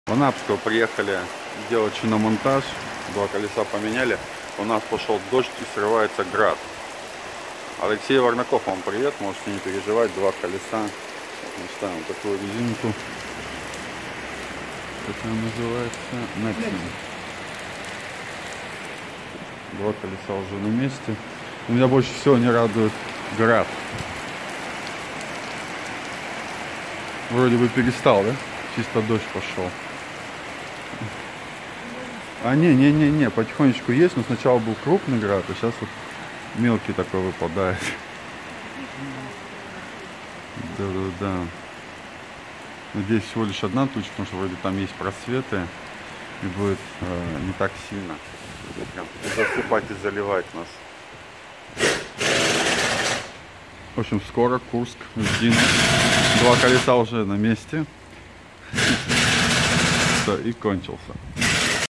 Анапа. Град. Меняем резину в Анапская. готовимся в дорогу 30.10.2017
Карта сбербанка: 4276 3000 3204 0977